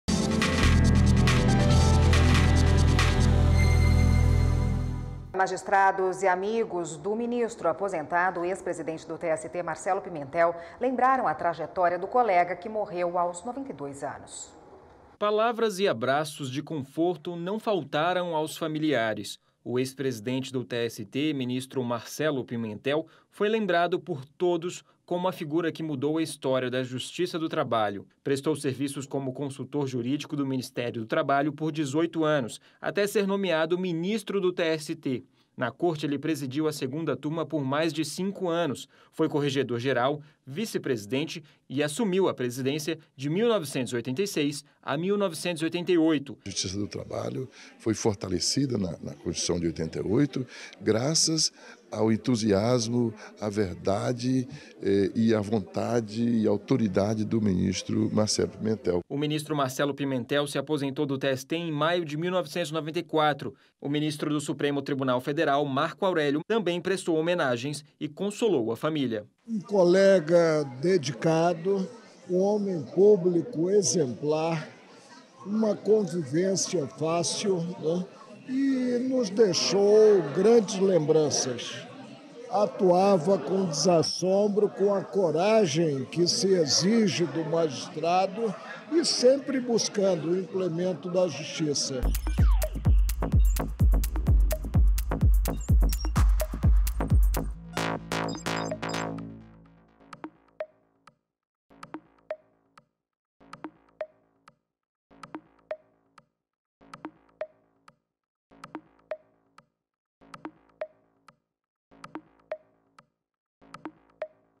O adeus ao ministro Marcelo Pimentel
Ministros, familiares e amigos se despediram do ministro aposentado do Tribunal Superior do Trabalho, Marcelo Pimentel que faleceu na quinta-feira (3) aos 92 anos.

Nascido o Vitória, Espírito Santo, o ex-ministro trabalhou com Tancredo Neves e Marcondes filhos. Prestou serviços de consultoria jurídica no Ministério do Trabalho por 18 anos até ser nomeado para o TST. Na corte, presidiu a segunda-turma e ocupou os cargos de corregedor-geral e vice-presidente, além de presidir o Tribunal entre os anos de 1986 e 1988.

O ministro Marcelo Pimentel se aposentou do TST em maio de 1994, e ainda exerceu o cargo de Ministro do Trabalho, no governo Itamar Franco. Depois atuou como advogado em Brasília.

O ministro Supremo Tribunal Federal, Marco Aurélio Melo também veio prestar suas homenagens e consolou a família “um colega dedicado, homem público exemplar, convivência fácil e nos deixou grandes lembranças, atuava com desassombro, com a coragem que se exige do magistrado, sempre buscando implemento da justiça. Agora digo sempre, que a pior morte não é a física, é a morte da fala, e sob o ângulo da fala, com as referencias positivas que podemos fazer, ele estará sempre entre nós “ disse.